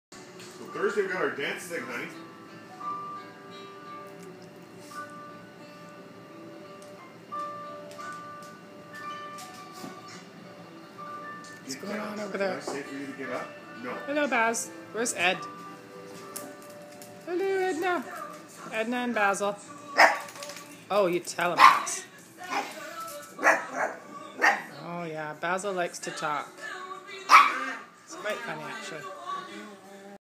Scooter Patooter - Tan & White Chi/Daschund mix boy for adoption
Scooter our adoptable little Chihuahua, daschund 1 yr old neutered male up for adoption through Size Small Dog Rescue - Langley, BC